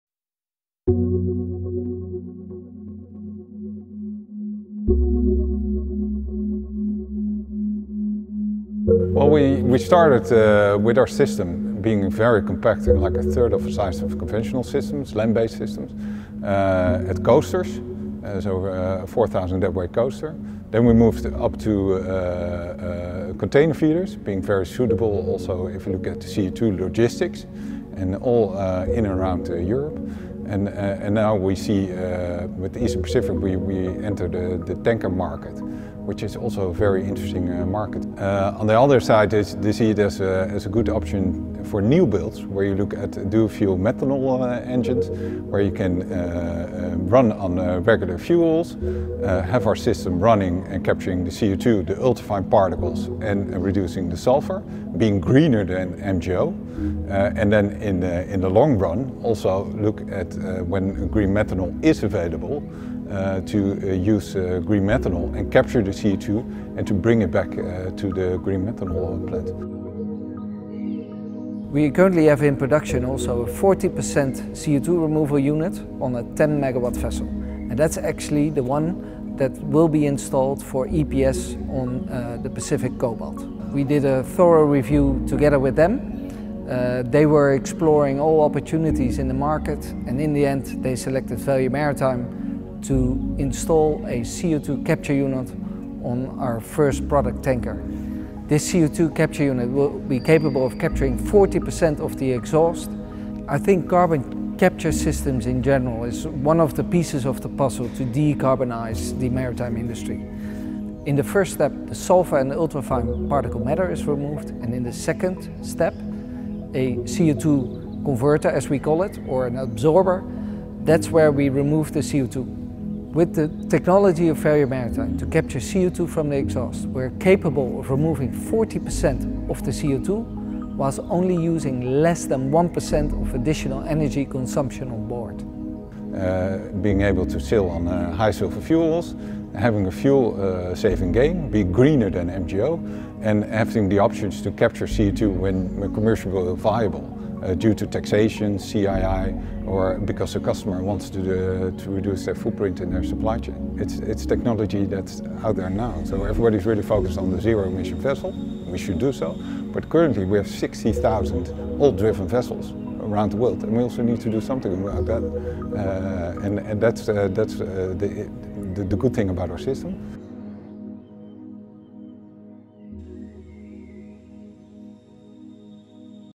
VM Interview - Signing with EPS for first carbon capture solution onboard tankers
Eastern Pacific Shipping (EPS) and Value Maritime (VM) have teamed up to install the first carbon capture solution onboard a pair of tankers.

The Singapore-based tonnage provider will retrofit two MR tankers with carbon capture systems, making them the largest ocean-going vessels fitted with carbon capture technology to date.

WM will install carbon capture and filtering systems on MR tankers M/T Pacific Cobalt and M/T Pacific Gold, with an option to equip three more vessels. The installation of the first system is scheduled to be completed within 2022 with engineering and planning underway.